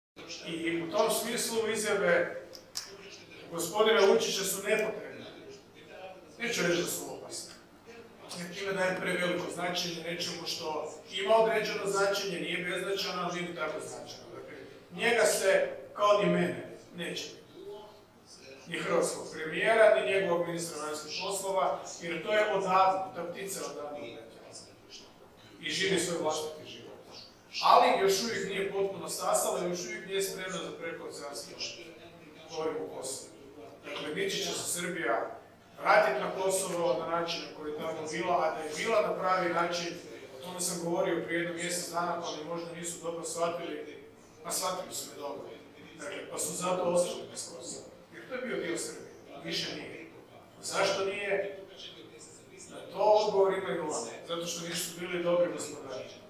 Milanović o Kosovu i Srbiji: Srbija se neće vratiti na Kosovo, nisu bili dobri gospodari
Predsjednik Republike Albanije Bajrama Begaja i prva dama Armande Begaj u drzavnom posjetu Republici Hrvatskoj kod predsjednika Republike Hrvatske Zorana Milanovića i prve dame Sanje Musić Milanović. Izjave za medije predsjednika Republike Hrvatske Zorana Milanovića i predsjednika Republike Albanije Bajrama Begaja.